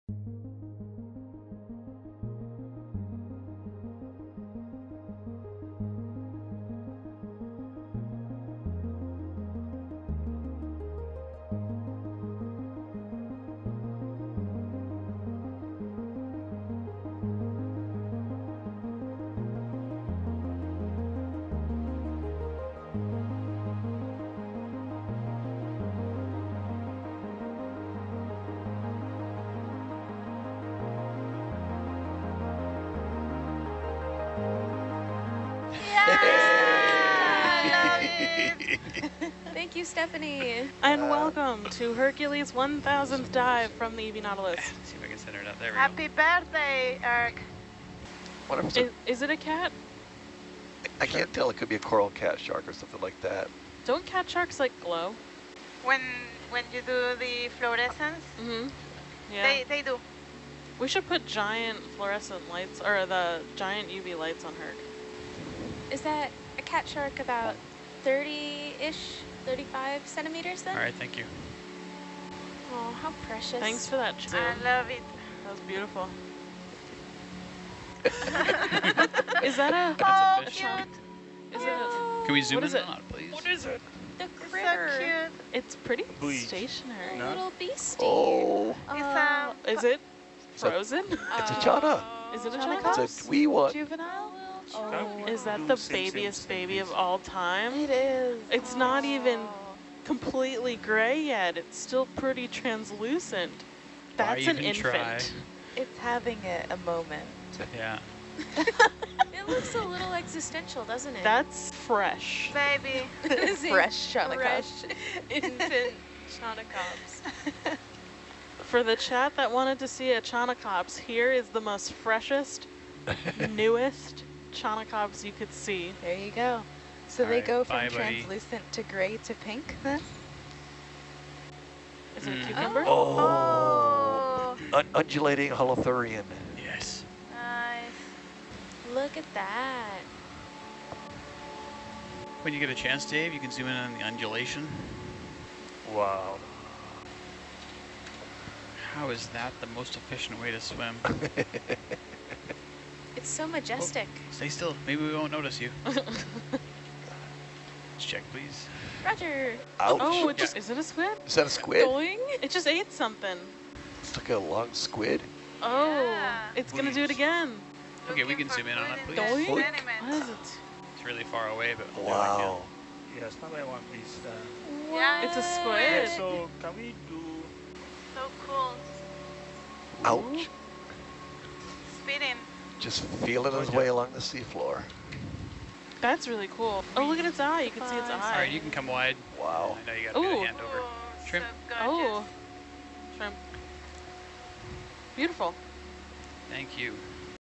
Wildlife and Wonder During a Milestone Dive for ROV Hercules | Nautilus Live
For the past 15 years, the Ocean Exploration Trust has explored the seafloor and shared discoveries live with the world. ROV Hercules has led our deep sea exploration, and this highlight video celebrates its 1000th dive from E/V Nautilus (which was preceded by many more prior to Nautilus)! Check out some of the awesome seafloor sightings from this celebratory dive that took place on August 24, 2023 about 10 nm east of Johnston Atoll during our NA153 expedition.

Witness a catshark and an adorable (likely) juvenile Chaunacops anglerfish as they meander in front of the vehicle's main camera. Watch an undulating Holothurian sea cucumber cause control room giggles and see a (tentatively IDed) Chiroteuthis sp. squid move above the seafloor. OET is proud to have celebrated this milestone while diving in these proposed marine sanctuary waters in and around the Pacific Remote Islands Marine National Monument.

Learn more about this expedition funded by NOAA Ocean Exploration via the Ocean Exploration Cooperative Institute